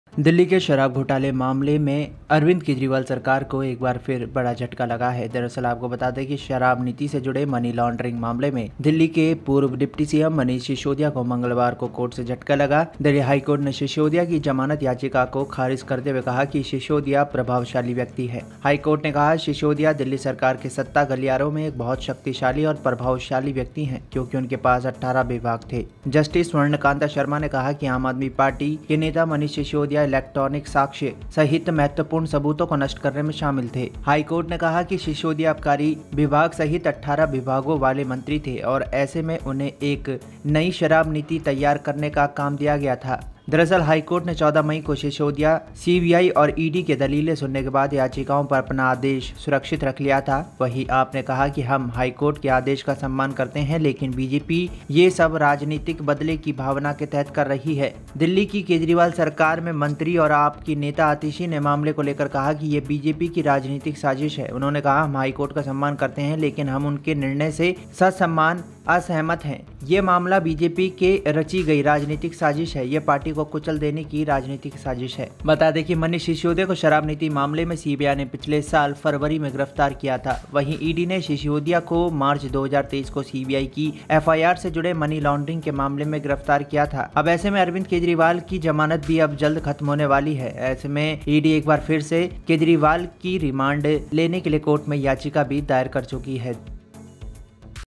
Delhi Excise Policy : HIGH COURT ने AAP Party को दिया झटका, Sisodia के उड़े होश, फसे Kejriwal
Delhi Liquor Policy: 'Manish Sisodia is very powerful because...', said the High Court while rejecting the bail plea.